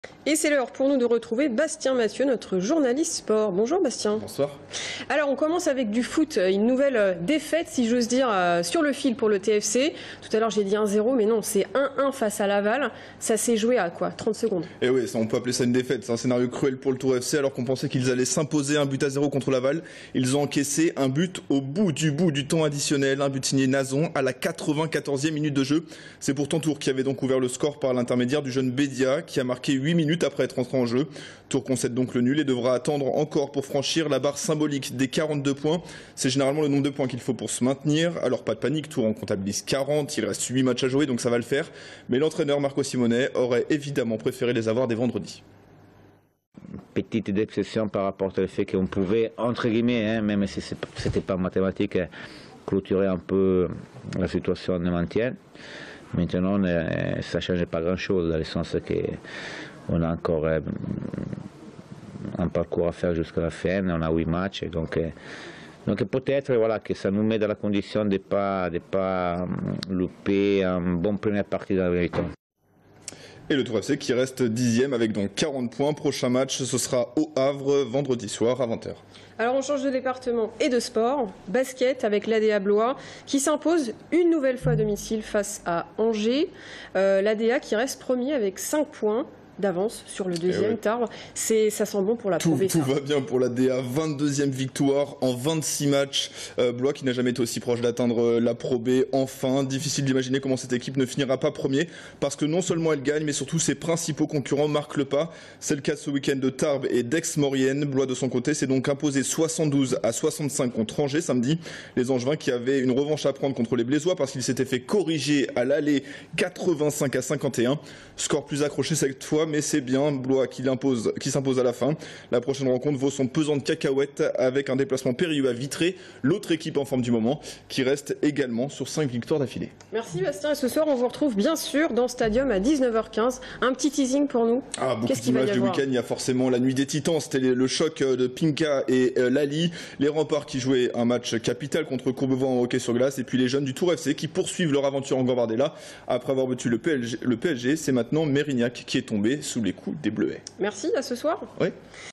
Sport: les temps forts du week-end
Sites webs/blogs, copiez cette vidéo avec le bouton "Intégrer" !
 Retour sur l'actualité sportive du week-end dans l'agglomération tourangelle.